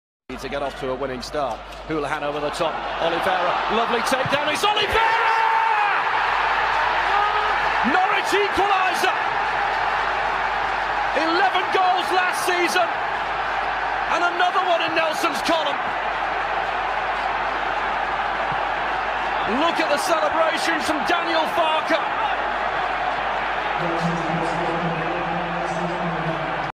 Nelson Oliveira - Angry Goal Celebration vs Fulham | Fulham vs Norwich City 1-1 | 05.08.2017
Nelson Oliveira started on the bench for Norwich against Fulham on Saturday at Craven Cottage and it's fair to say he wasn't happy about it judging by his celebration after scoring the equaliser late on.

Oliveira approached manager Farke pointing to his name on the shirt clearly trying to make a point and then continued after his manager had given him a hug.